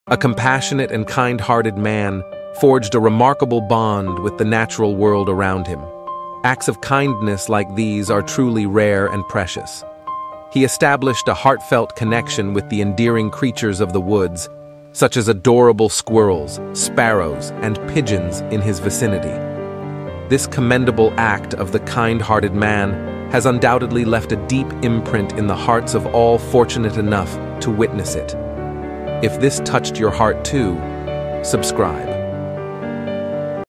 The Man Who Became Friends with Squirrels and Bird ❤️
A compassionate and kind-hearted man forged a remarkable bond with the natural world around him. Acts of kindness like these are truly rare and precious. He established a heartfelt connection with the endearing creatures of the woods, such as adorable squirrels, sparrows, and pigeons in his vicinity.
Life is often too busy, but the moments he shared with these animal friends served as a lesson in compassion and respect for all forms of life. This commendable act of the kind-hearted man has undoubtedly left a deep imprint in the hearts of all fortunate enough to witness it.